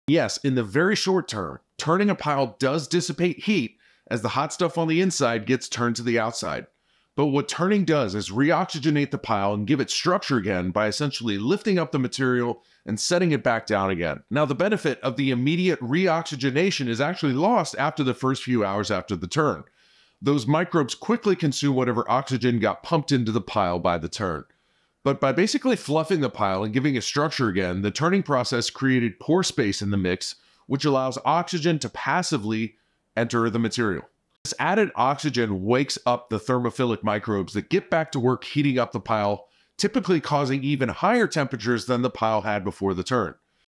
Turning a Compost Pile. What Happens?
Does turning a compost pile heat it up or cool it down?